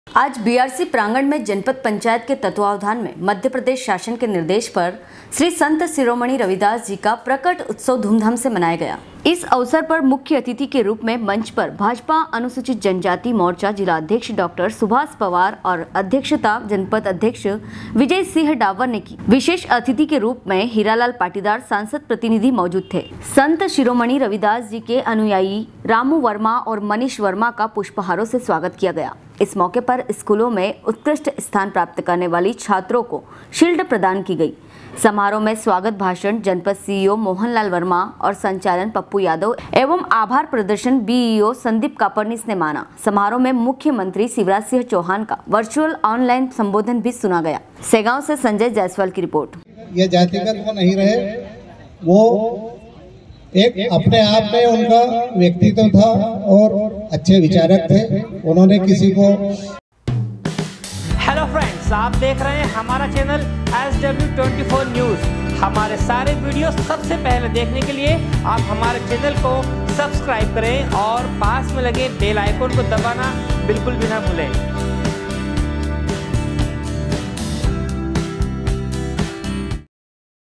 SW 24 News Madhyapradesh
आज बीआरसी प्रांगण में जनपद पंचायत के तत्वावधान में मप्र शासन के निर्देश पर श्री संत सिरोमणी रविदास जी का प्राकट उत्सव धूम धाम से मनाया। सेगांव से संजय जायसवाल की रिपोर्ट